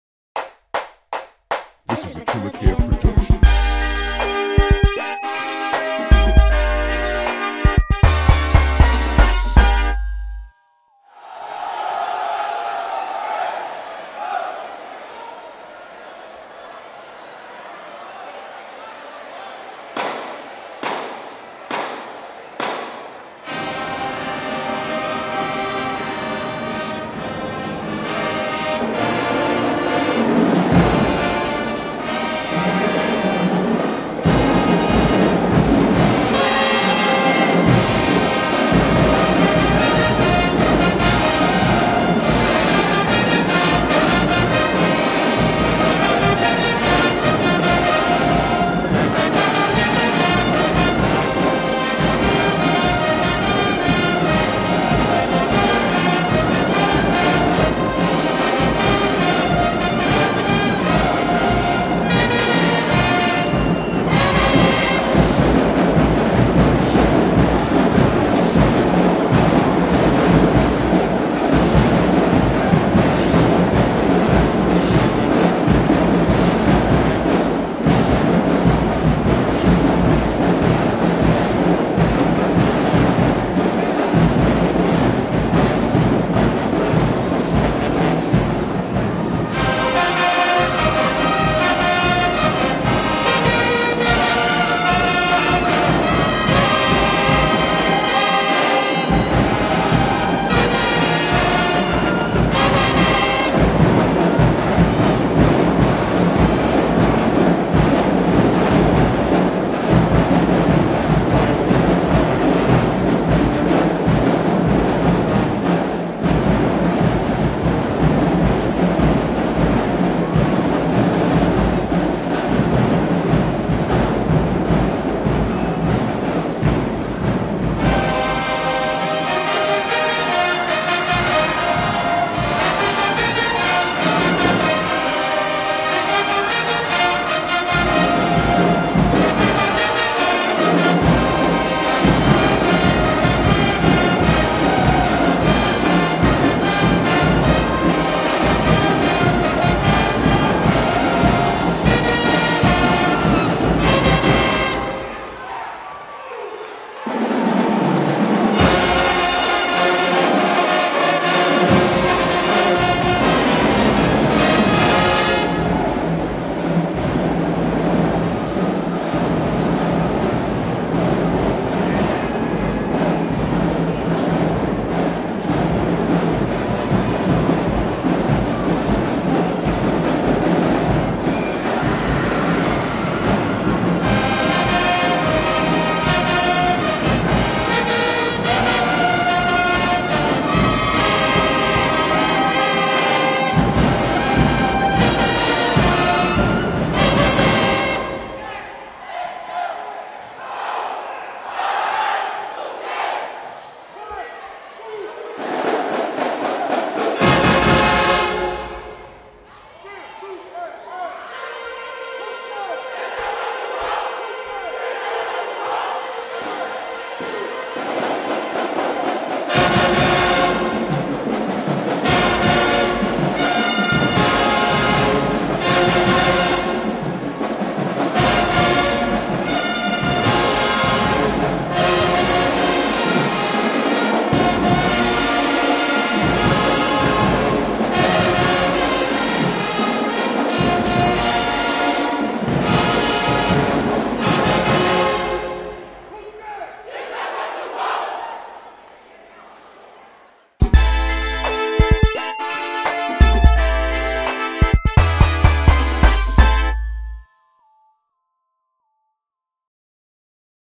Jefferson Davis v.s. Selma High School Showdown - Round 2 - 2016
The Selma High School Marching Saints host their first annual "Battle of Highway 80" vs The Jefferson Davis High School Marching Vols in this 2016 Showdown in Selma, Alabama.